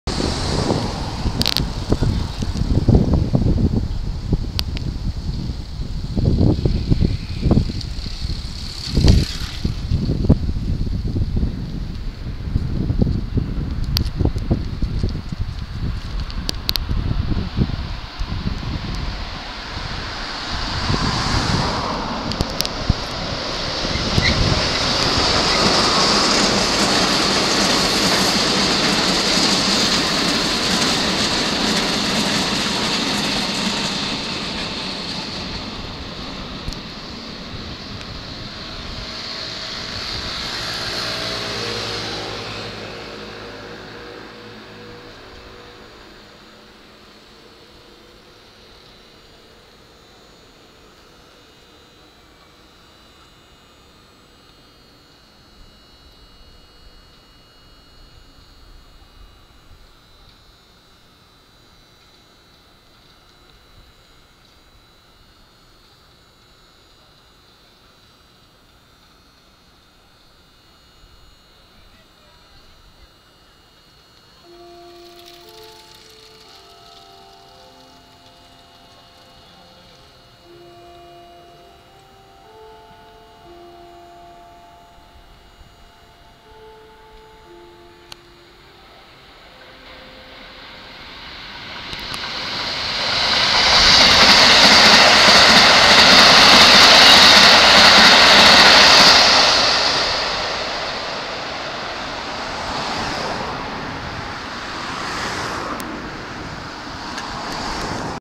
京急線99S特急金沢文庫行き1409が金沢文庫検車区に停車中！京急線普通浦賀行き1355と京急線3A快特品川方面泉岳寺行き2148が走行！【金沢文庫駅～金沢八景駅、令和4年11月27日日曜日】
京急線金沢文庫駅～金沢八景駅間から撮影。

撮影器材UJIFILM FinePix XP120

※サムネイルの写真の撮影者も猫ぷ(令和4年11月27日日曜日12時44分撮影。)

金沢文庫駅～金沢八景駅

金沢文庫検車区に停車中及び金沢文庫駅～金沢八景駅間を走行中の、
京急線4両編成99S特急金沢文庫行き1000形409～412編成けいきゅん

京急線下り6両編成普通浦賀行き1000形355～360編成

京急線上り8両編成3A快特品川方面泉岳寺行き2100形48～41編成けいきゅん
の京急線の電車。